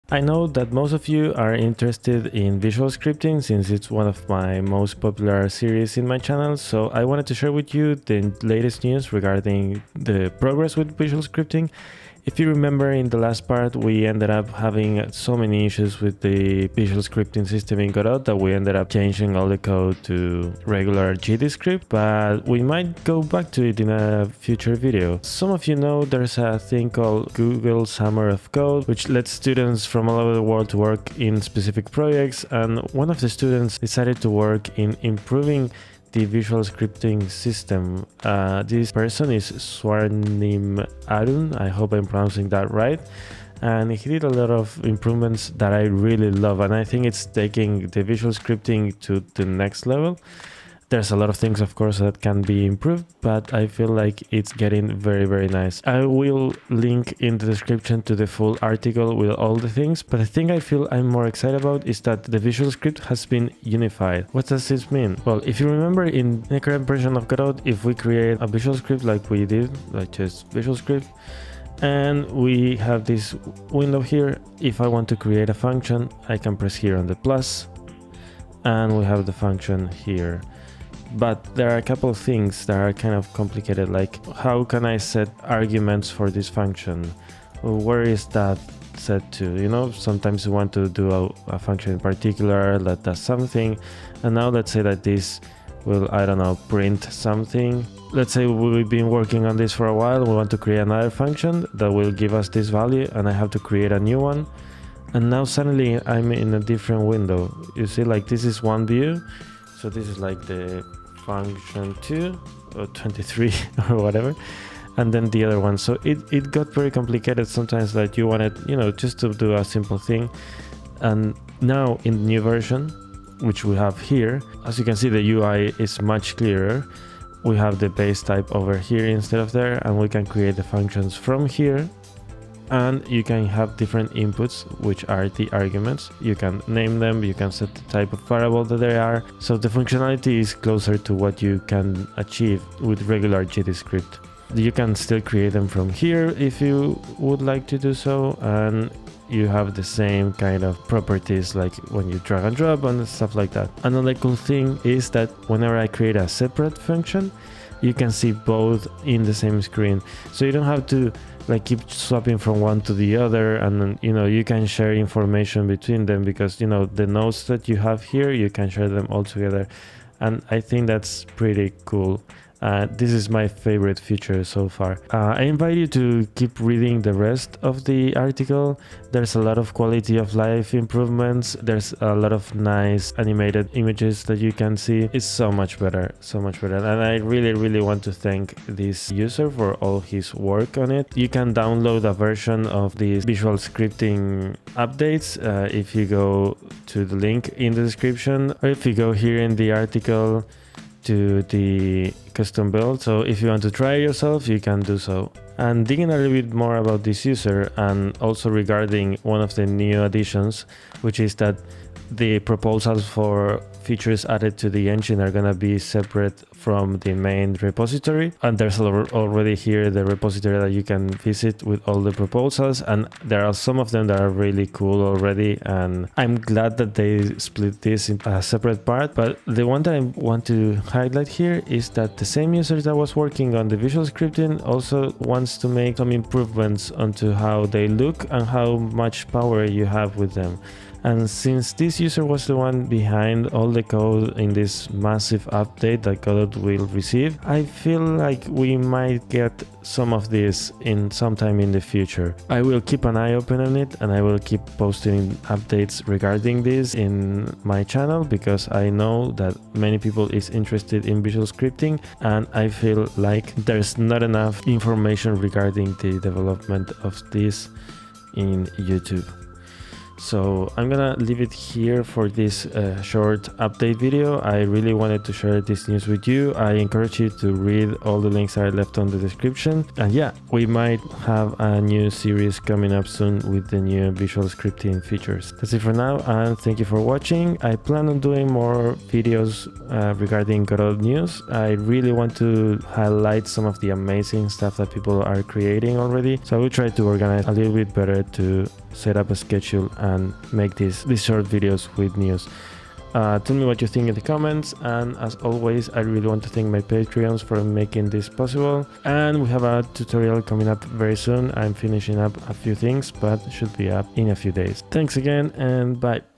Visual Scripting News!
There is an amazing update coming up to Visual Scripting in Godot! I made a video covering a bit about it and my favorite feature.